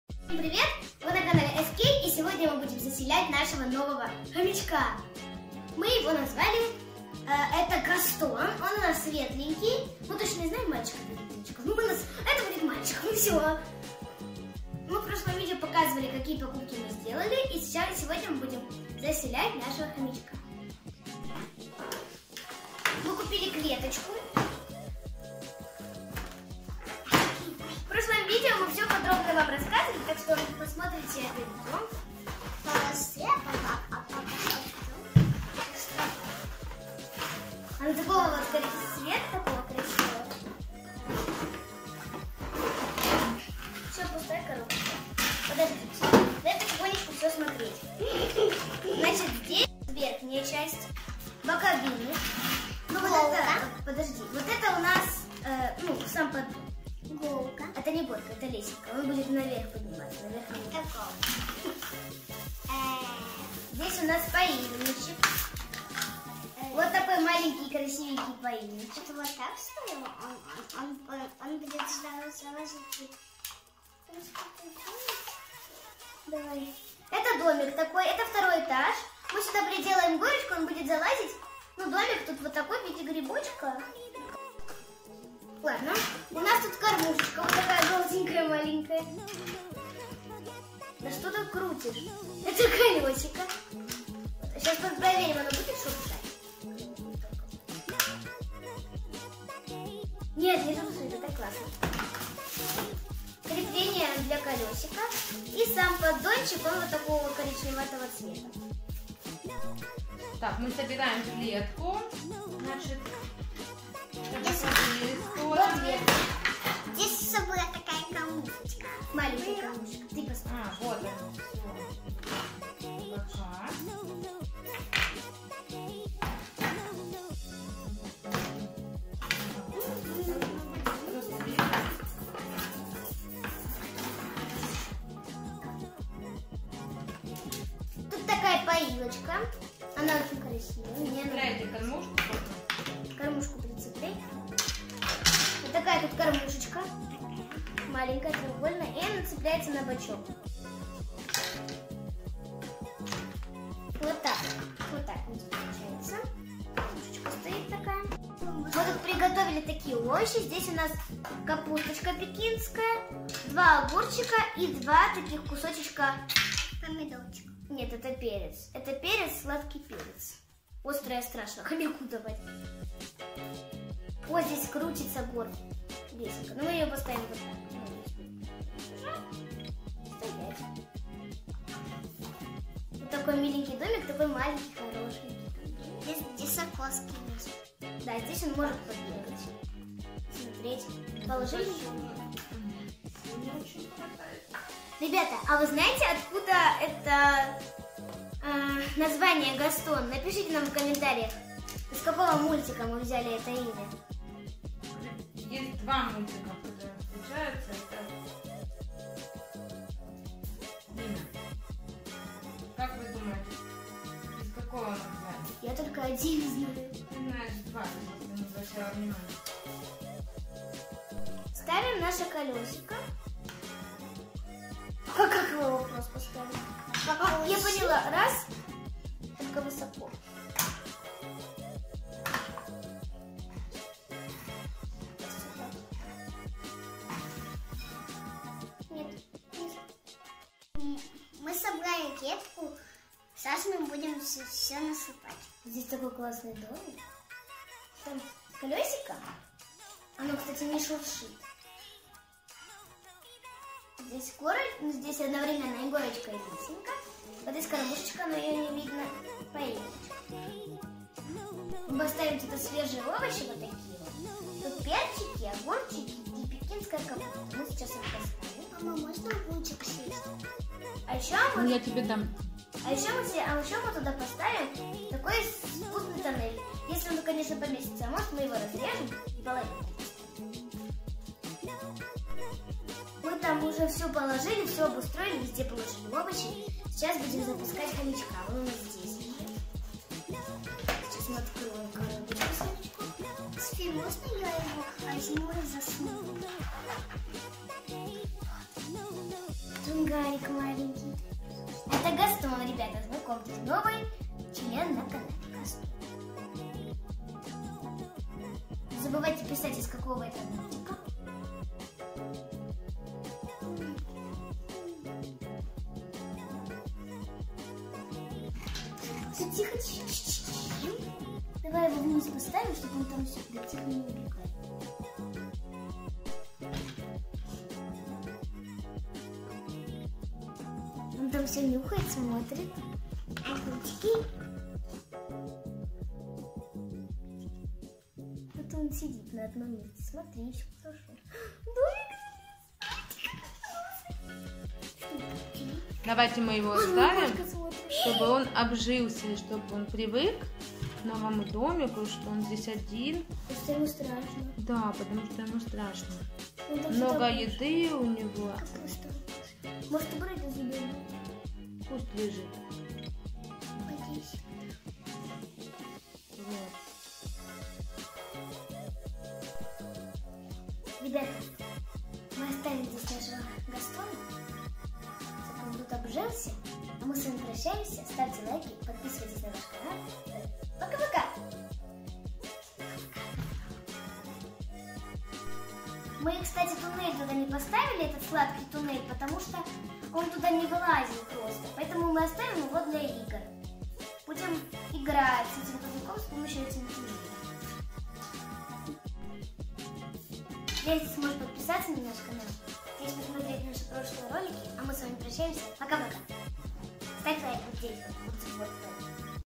КАК СОБРАТЬ КЛЕТКУ для ХОМЯКА | ЗАСЕЛЯЕМ ХОМЯКА В ДОМИК
Привет! Вы на канале ЭСКЕЙ! Пришло время собрать домик для хомячка и заполните его клетку. Была несложная инструкция и мы быстро справились с этим домиком. Наверно хомяку он понравится. ПОехали!))

Hello! You are on SK channel! It's time to build a house for a hamster and fill his cage. There was a simple instruction and we quickly coped with this house. Probably the hamster will like it. Go!))

0:10 -  Всем привет
1:28 -  Вот что у нас есть
2:32 -  У нас начинает получаться
4:09 - Хомяку понравится эта клетка?